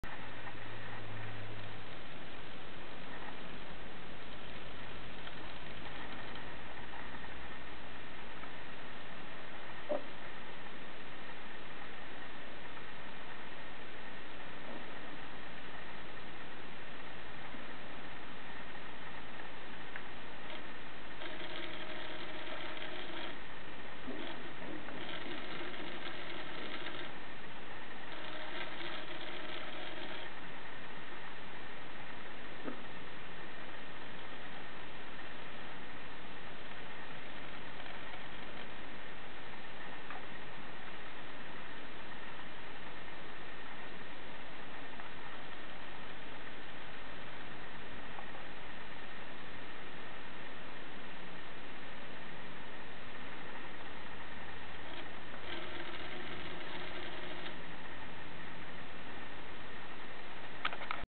Motorized MENCK M154 LC excavator (N scale) / Motorizovaný bagr MENCK M154 LC (1:160)
I was motorized MENCK excavator by using 3 model servos. This is first beta version for test of moving. Excavator is from Kibri 19101 construction set. Not painted yet.

Pomocí tří modelářských serv jsem si motorizoval MENCK bagr. Tohle jsou první pohybové testy. Bagr pochází ze stavebnice Kibri 19101; zatím není správně nabarven.